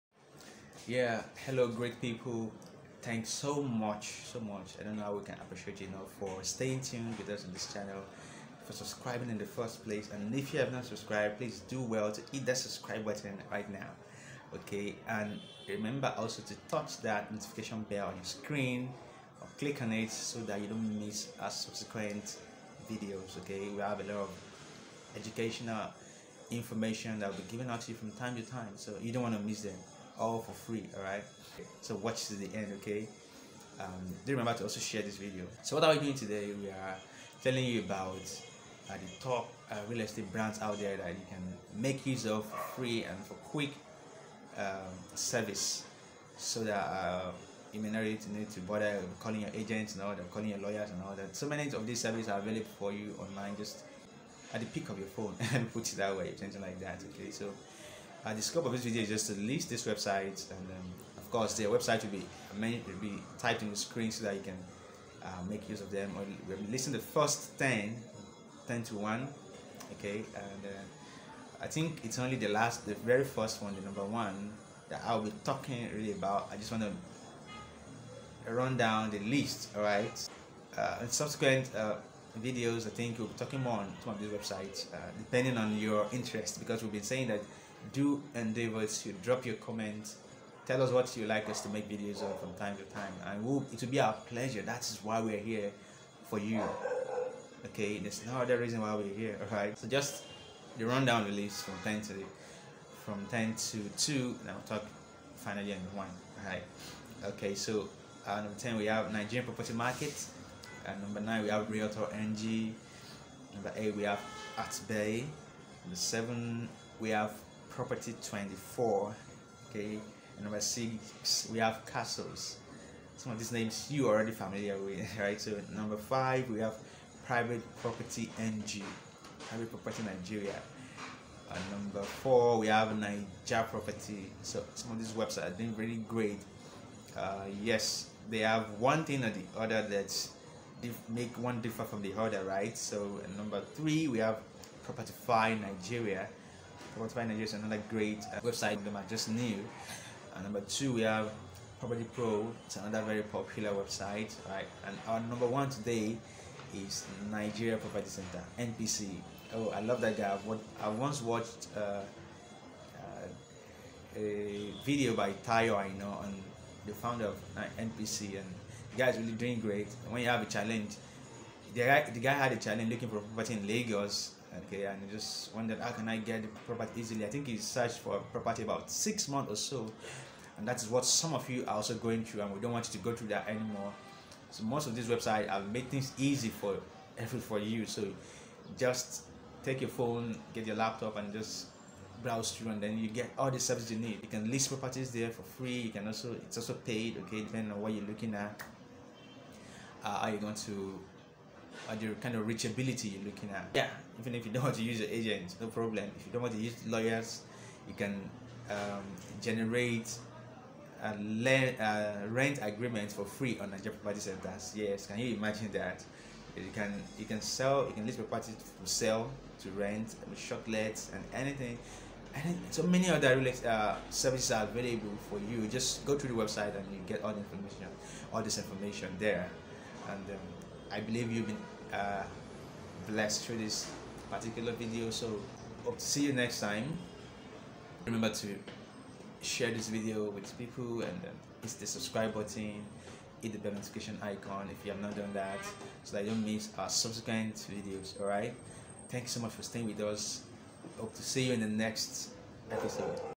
TOP 10 PROPERTY LISTING WEBSITES IN NIGERIA 2021 With Realtor Olusegun Richard
Use this information in a DIY approach to get needed real estate service from the privacy of where you are...